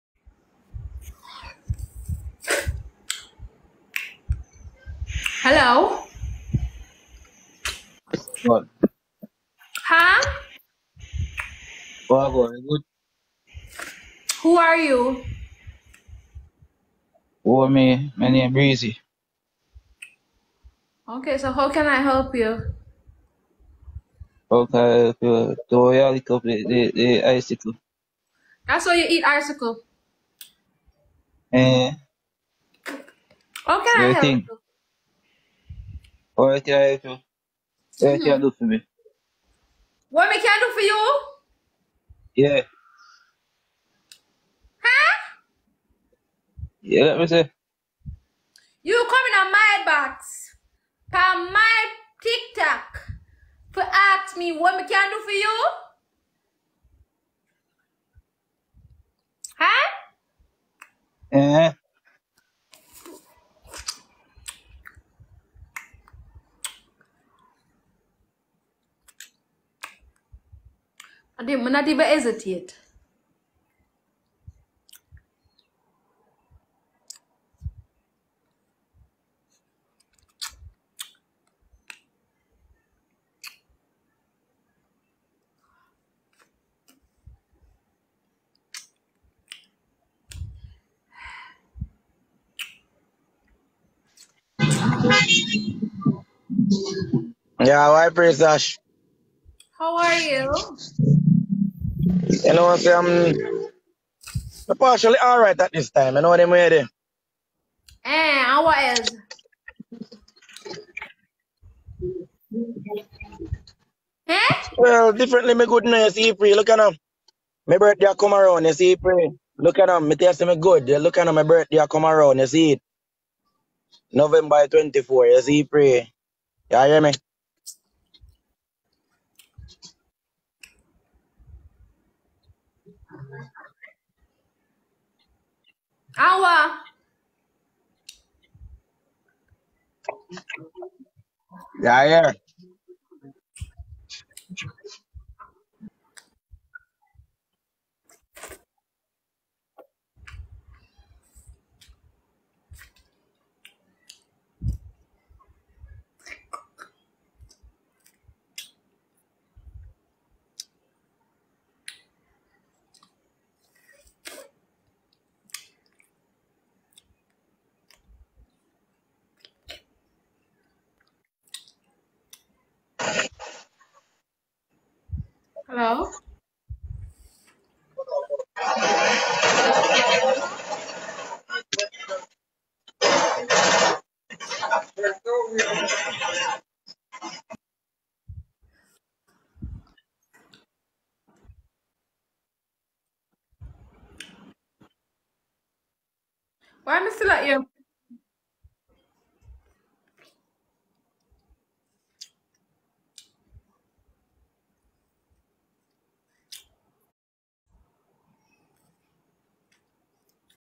Sashana Gapeteaser tasting popsicle like 😍 it's running while talking that's sweet Real Live Video
The video is about a person who does Tik Tok lives by preying on the desires of men, she is a Jamaican who speaks dirty and flirty with men. her name Sashana Gapeteaser  because of her teasing ability but for this video she takes out the sweet stuff and start dealing like a pack of cards wow. .